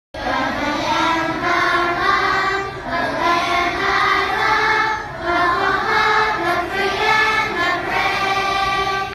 The 14th annual Veterans Day Salute at LaLiberte Elementary School
By: mvasconcellos
Published on: November 10, 2016